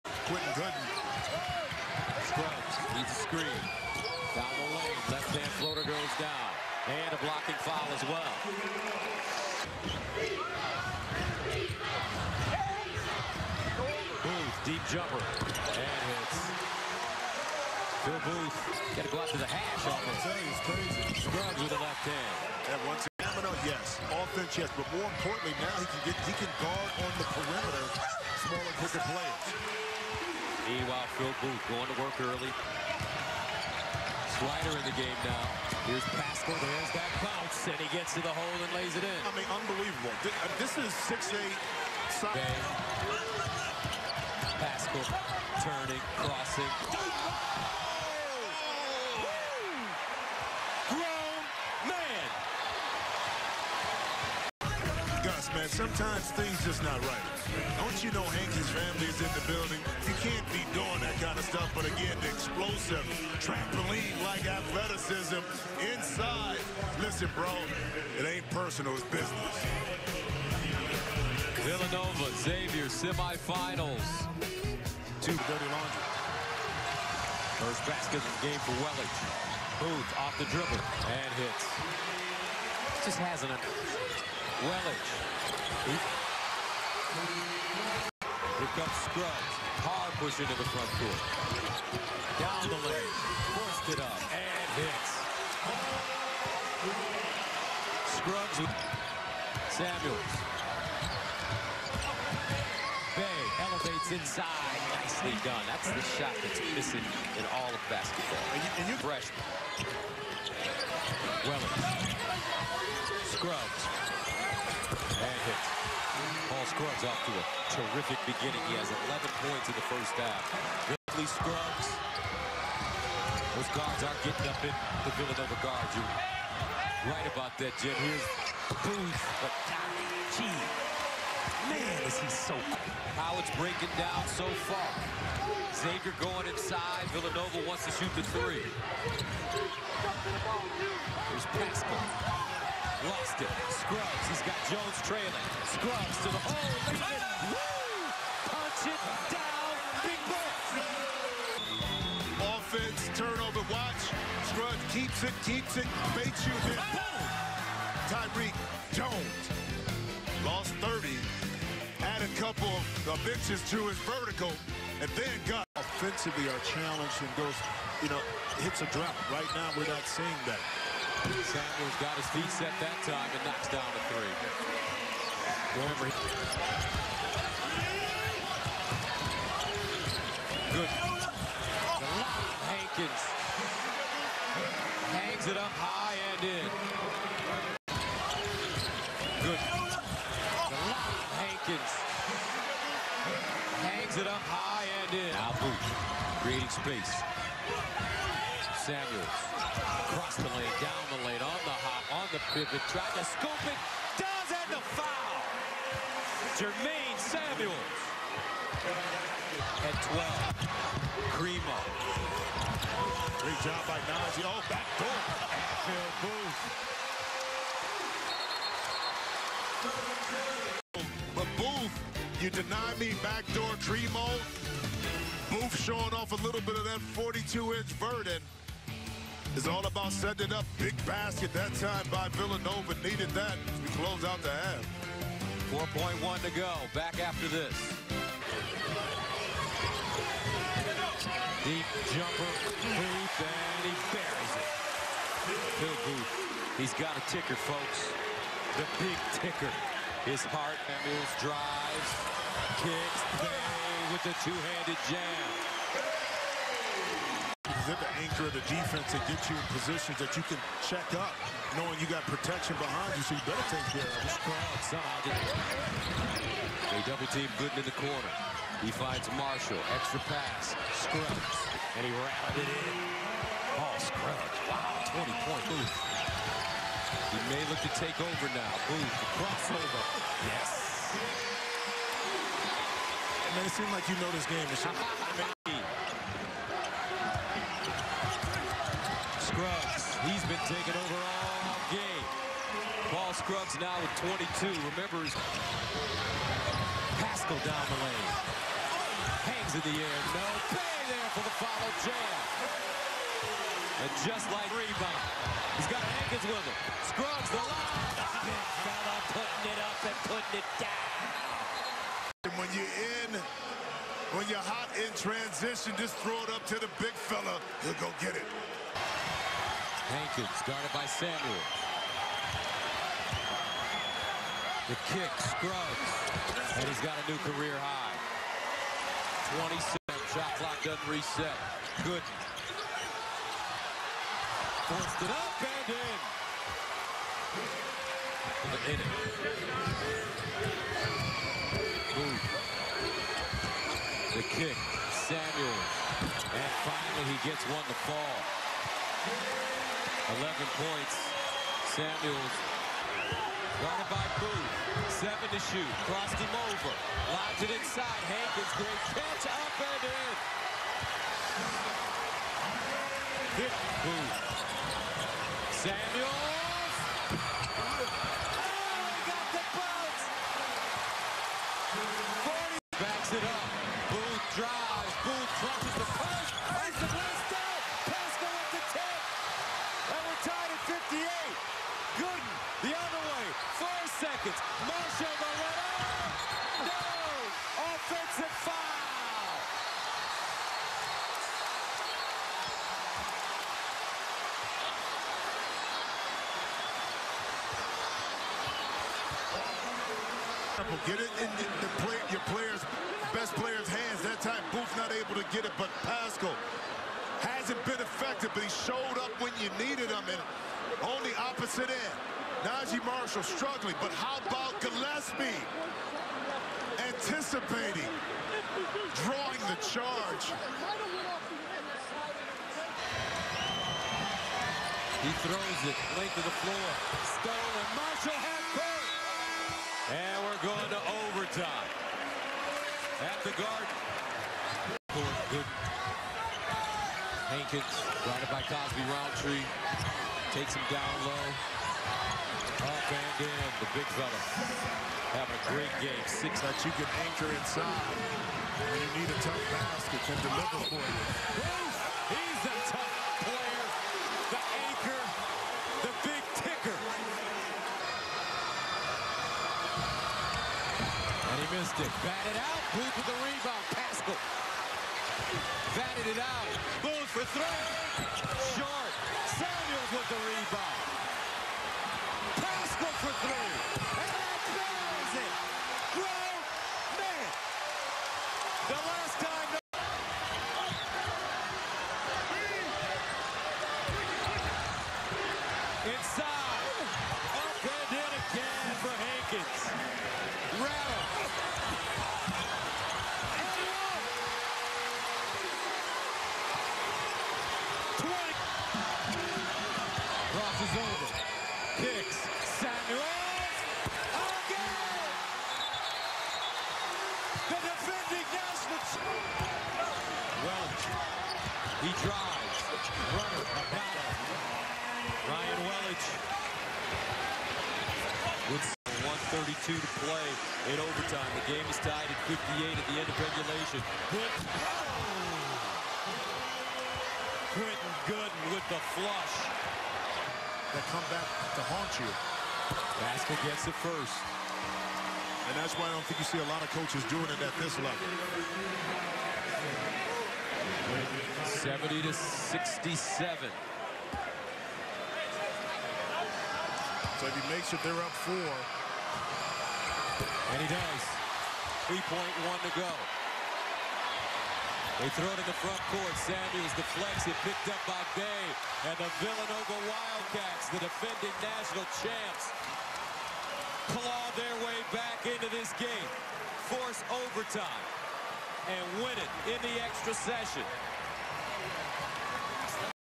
Villanova vs. Xavier Highlights
#1 Villanova - 71
#4 Xavier - 67
March 15, 2019
2019 BIG EAST Tournament Semifinals